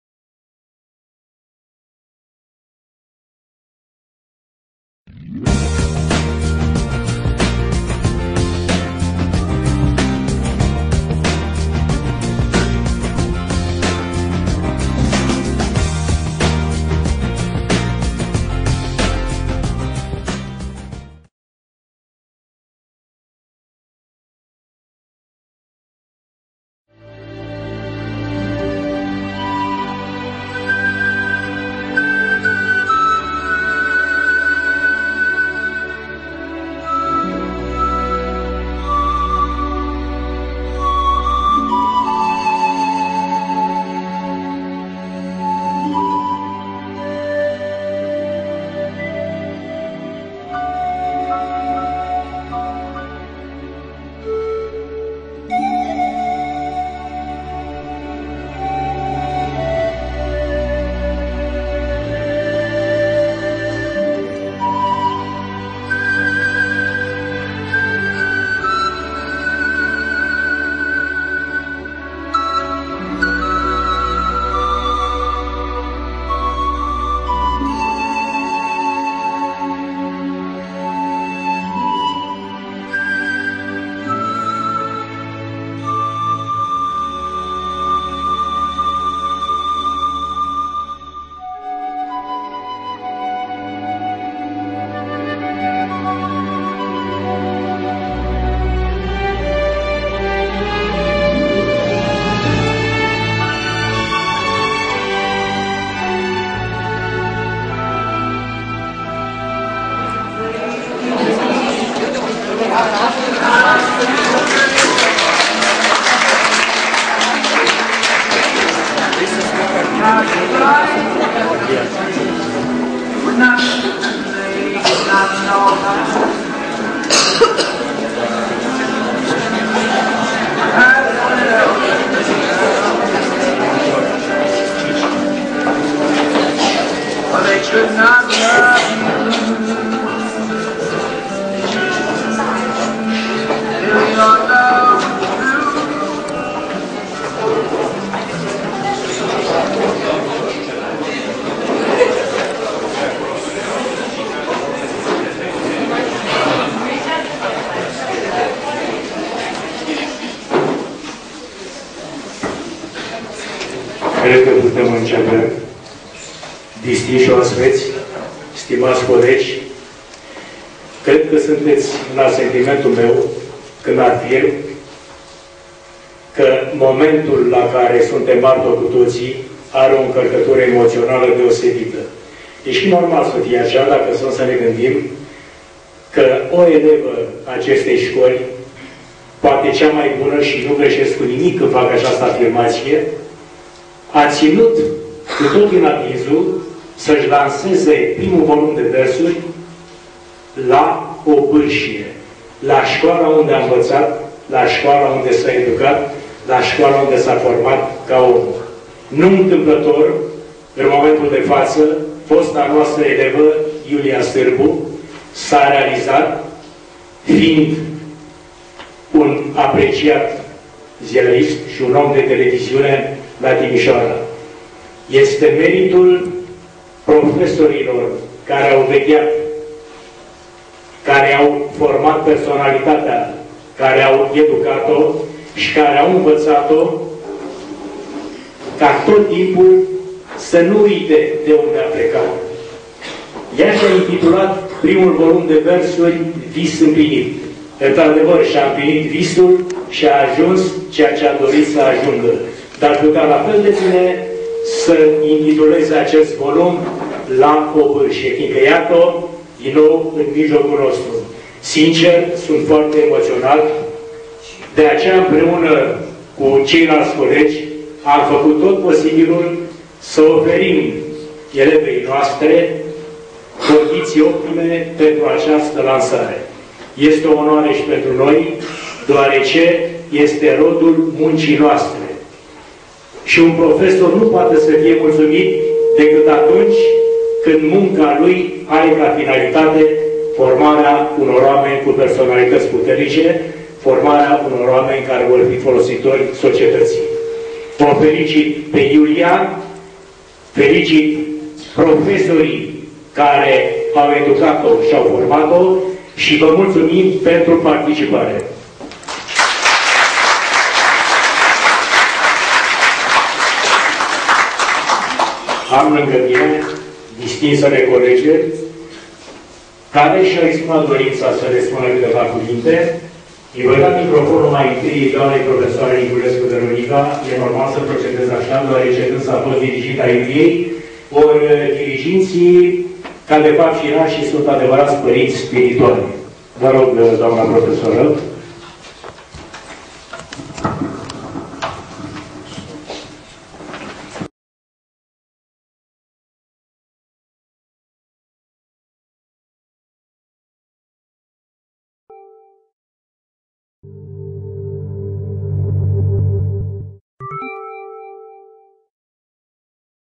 Iulia Sirbu - Vis implinit - volum de poezii
Vis implinit, de Iulia Sirbu, Calafat 2006
Discursul domnului director Bora Dan
Gr. Sc. Stefan Milcu